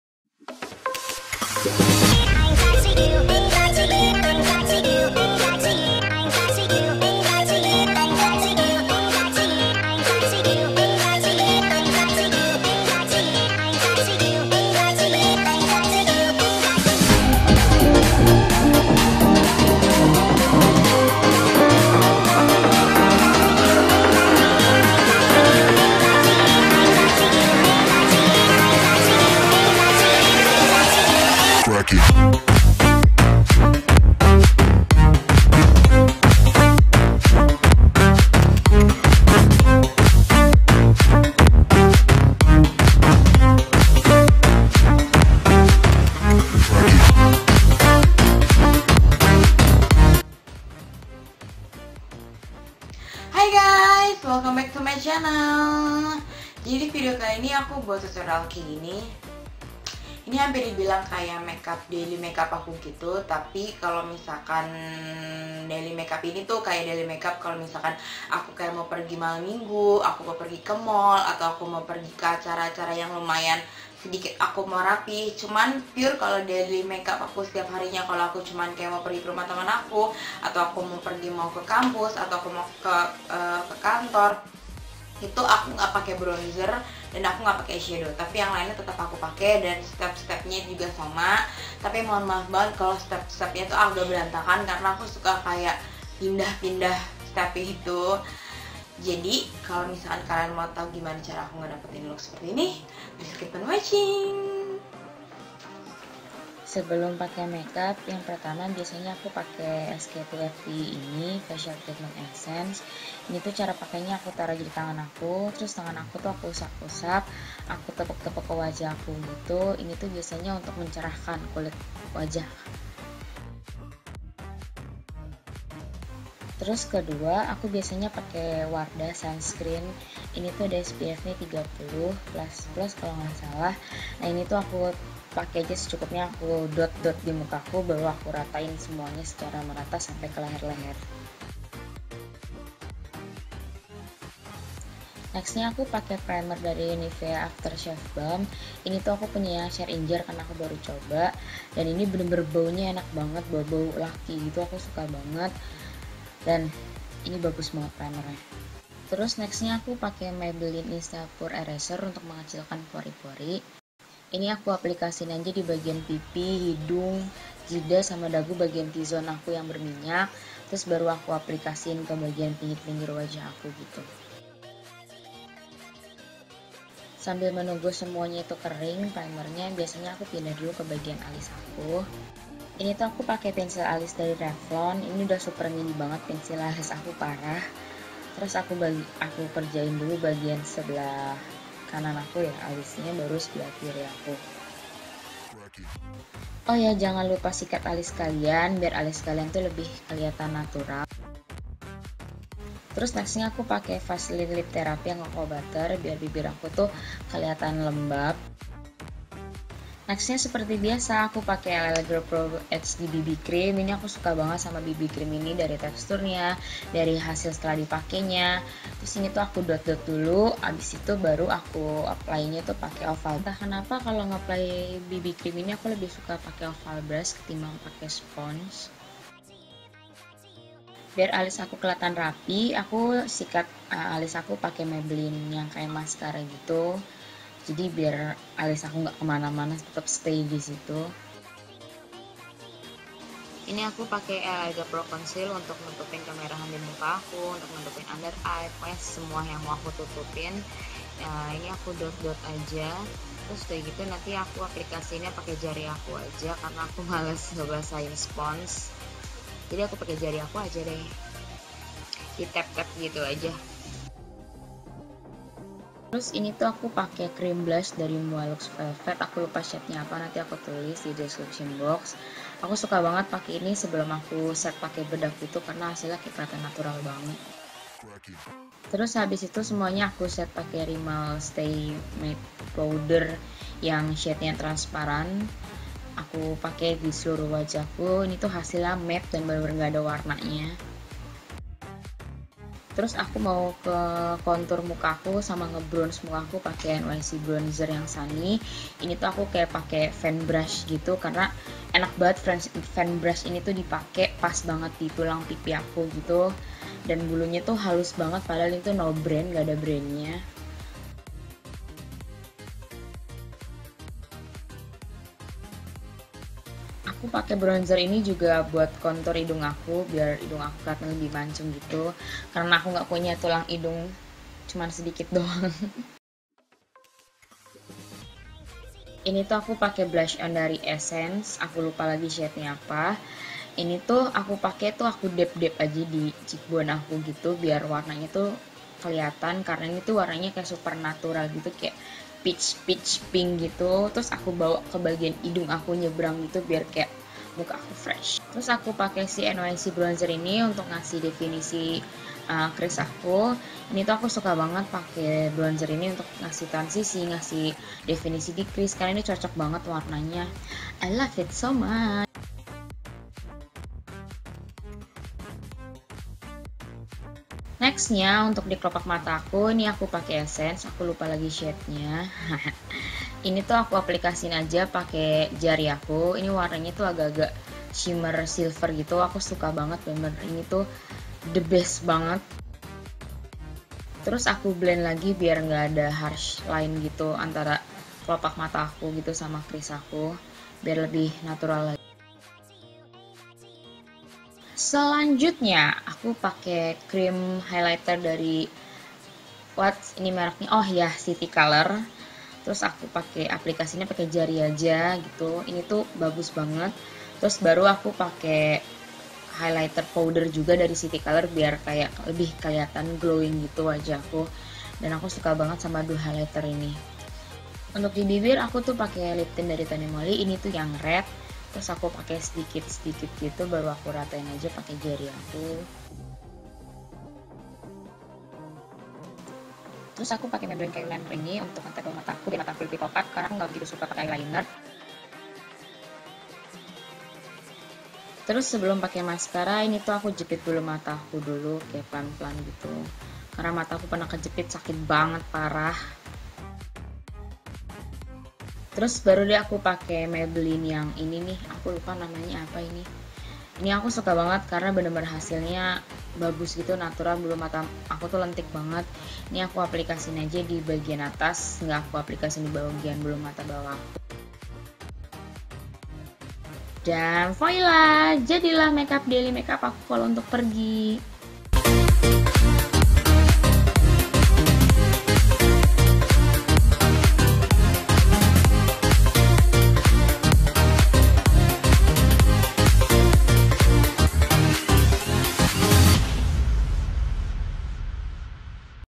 Special Daily Makeup routine // shabrinamanda
ENJOY FOR WATHING .

PRODUCT :
* sk-ii facial treatment
* Wardah suncreen gel spf 30++
* Nivea after shave balm (share in jar)
* Maybelline baby skin instant pore eareser
* Revlon eyebrow dark brown
* LA girl pro HD bb cream (LIGHT)
* Rimmel staymatte press powder transparant
* NYC bronzer Sunny
* Essence metal glam eyeshadow
* Maybelline hypercurl volume express
* City color glowing complexcion
* city color intese exposure highlighting
* Tony Molly delight tint (RED)

SONG :
By NCS REALEASE

XOXOXOXOXOX